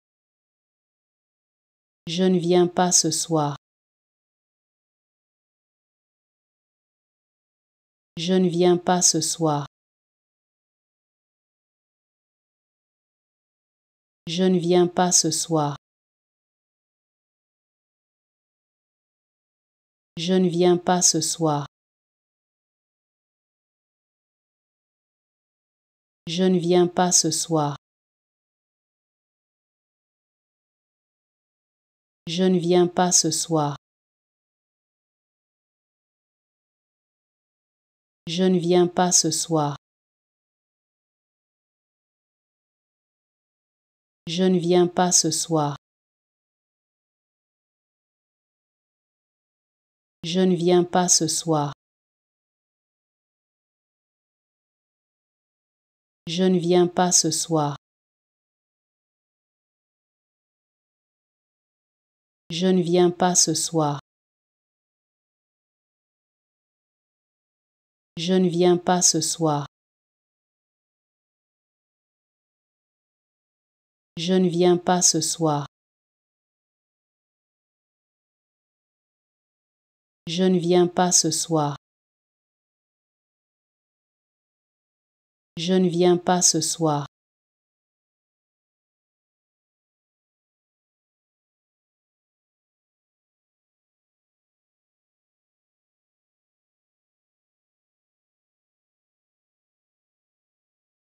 How to Pronounce ''Je ne viens pas ce soir'' (I am not coming tonight) Correctly in French
Learn how to say and properly pronounce ''Je ne viens pas ce soir'' in French with this free pronunciation tutorial.